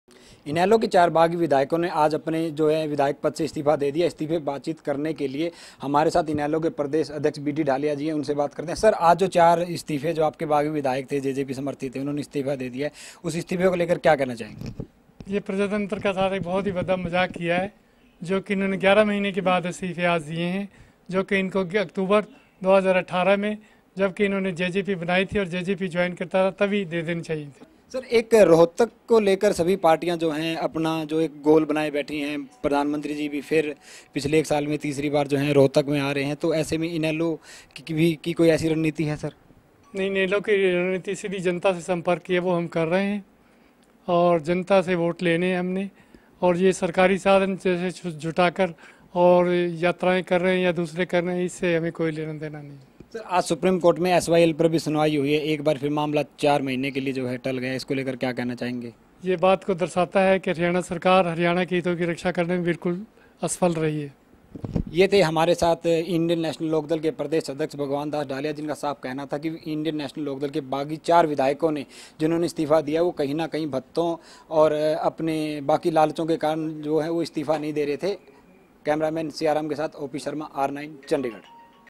इनेलो के 4 बागी विधायकों का इस्तीफा
इंडियन नेशनल लोकदल के 4 बागी विधायकों ने हरियाणा विधानसभा अध्यक्ष को अपना इस्तीफा सौंप दिया...जिसको लेकर इनेलो के प्रदेश अध्यक्ष बीडी ढालिया ने कहा कि इन लोगों का इस्तीफा लोकतंत्र के लिए एक मजाक है...ये सभी ने सरकारी भत्तों और ग्रांट के लालच में अपने पदों से पहले इस्तीफा नहीं दिया..